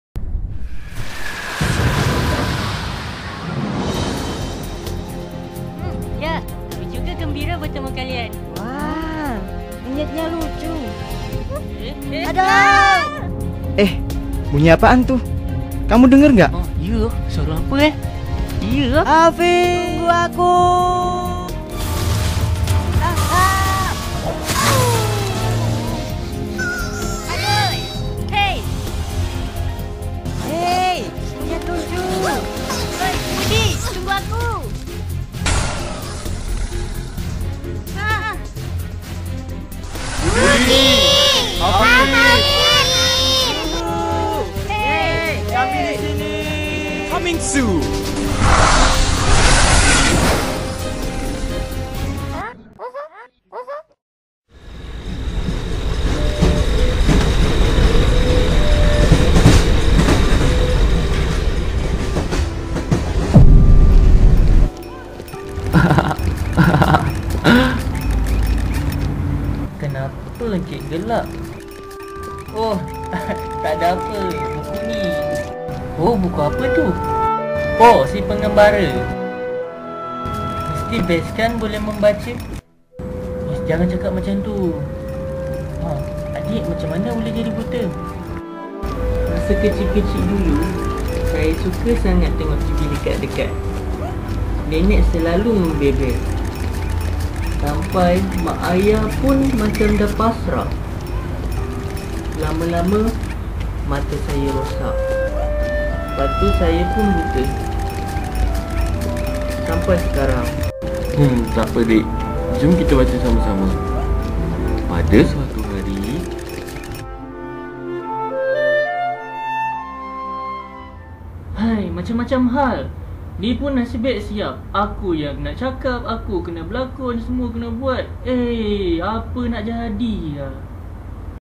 Abandoned animation - Bit Si Buta (subtitle available)
First animation trailer entitle Arti persahabatan. my roles- Director, Character designer, animator, voice over.

The 2nd animation:
tells about the situation at the train station where a blind boy who is jealous of the strangers sitting beside him who sounded so amused of reading book, then the conversation took place between them, bit tells how he became blind because of too much watching tv.

i never publish this animation anywhere, because me personally dont like the quality, it took me a week to complete the animation and a month for the pre-production. If i can spend more time on this, im sure it will be way better.